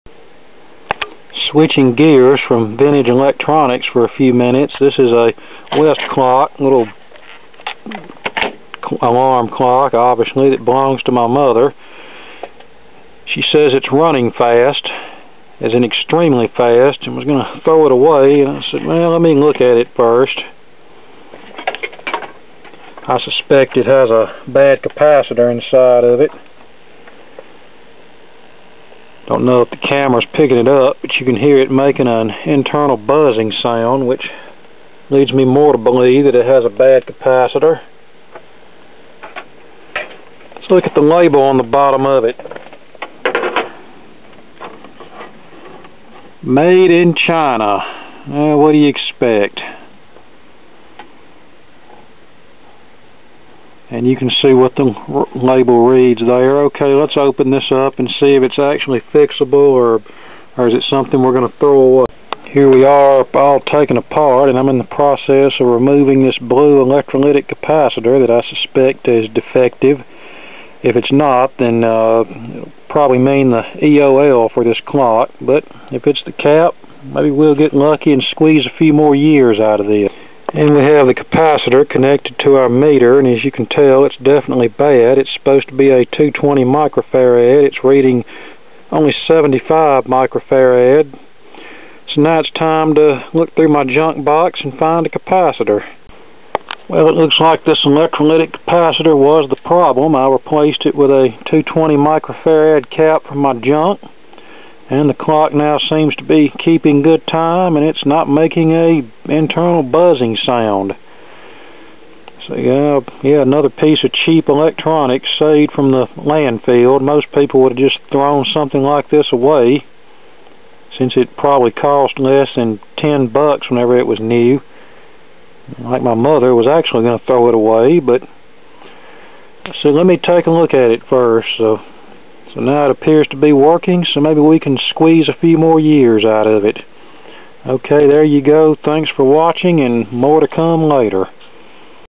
Repair of a cheap Chinese Westclox digital clock
OK, I'll admit that this is probably the most boring of any video that I've made.

This is a Westclox digital alarm clock, probably from the '90's, that belongs to my Mother. Recently, it started running very fast and was making a buzzing sound. Rather than throw it away, I opened it up to find a bad 220uf electrolytic cap and the clock now runs fine after replacing the cap with one from my junk box.

This clock probably cost under $10 when new and most people would have thrown it out; but, being the "ultimate electronics recycler" that I am, I'd rather fix it rather than waste $10 on a new one.